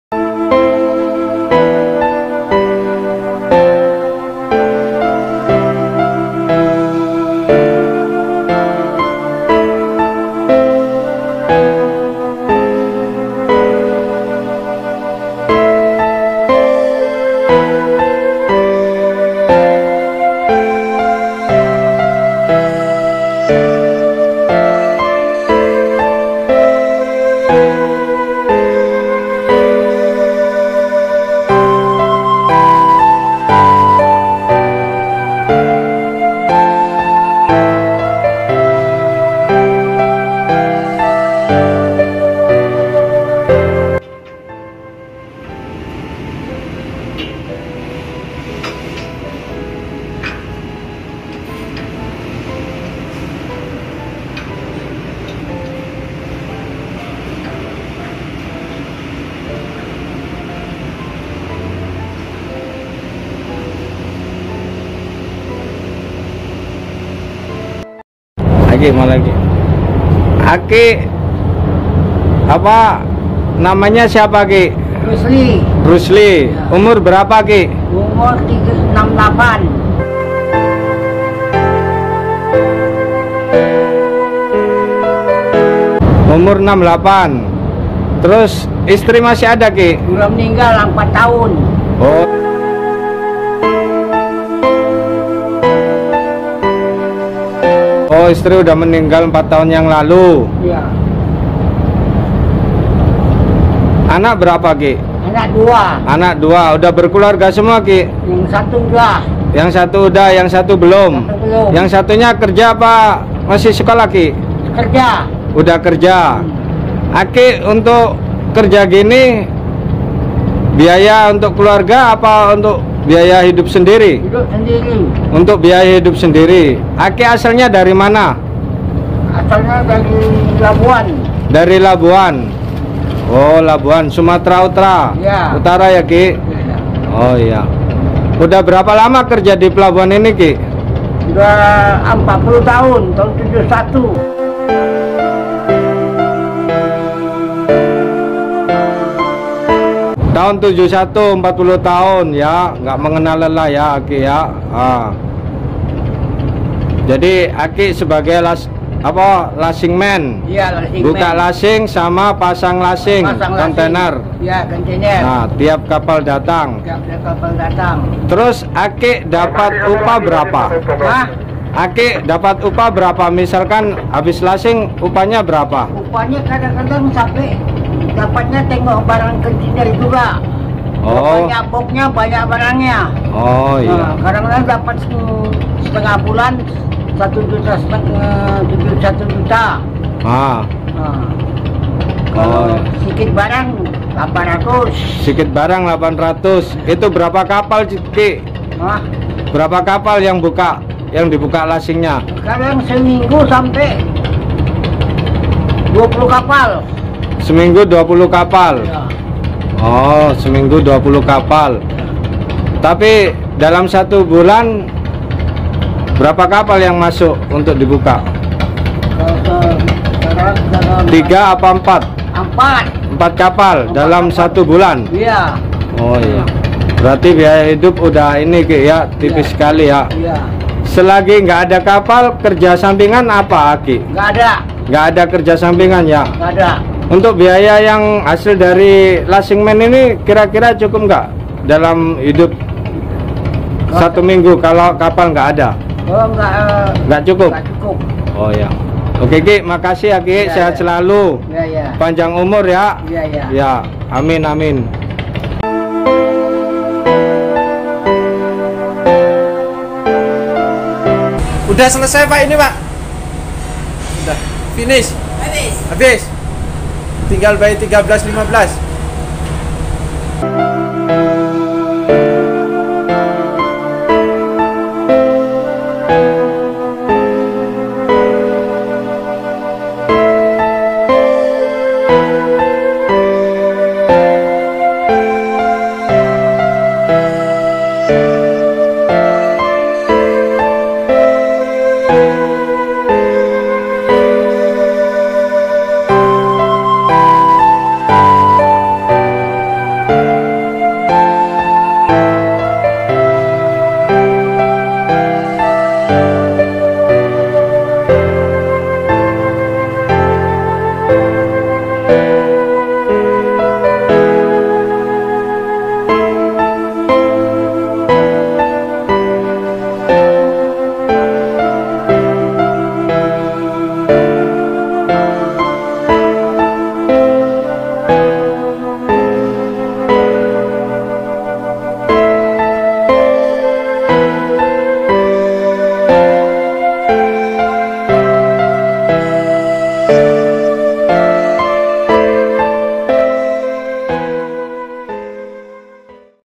KAKEK SUPER KUAT || Lashing man pelabuhan contener Belawan.
walaupun usia 68 tahun pak RUSLI memiliki semangat kerja yang tinggi untuk mencukupi kebutuhan sehari - hari.
kita doakan pak Rusli sehat selalu, selalu di lindungi dalam pekerjaannya amin